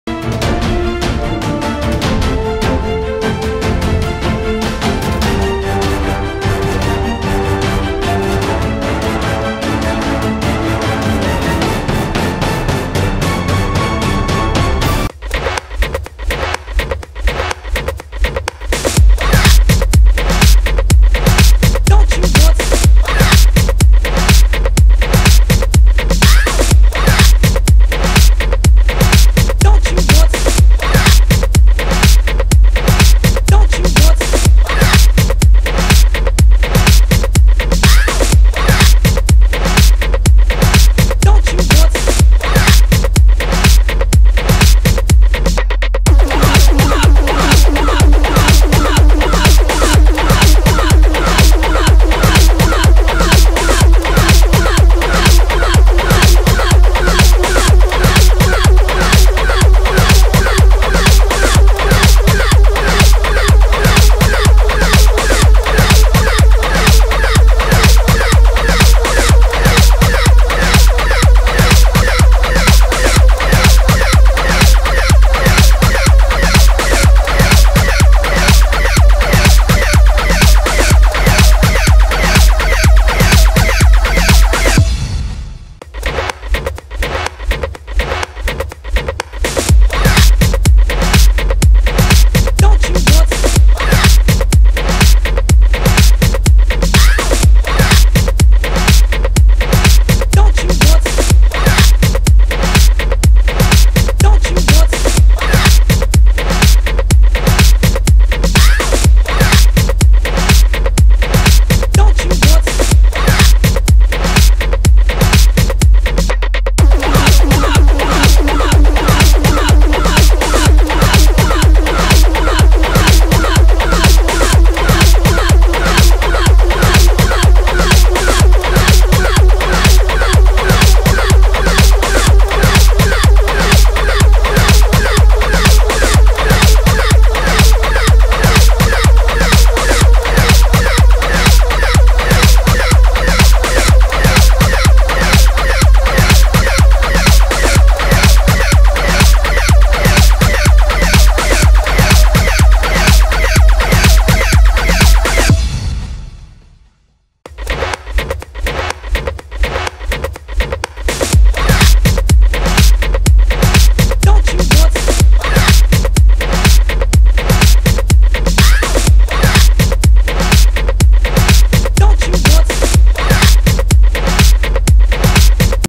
Car Wash Hot Girls Dancing - Tuning Show Video in Rome
Amazing car tuning show video: two beautiful and hot girls danncing to wash a car by White Angels Team. Video recorded in Guidonia, near Rome,at H24 Restaurant.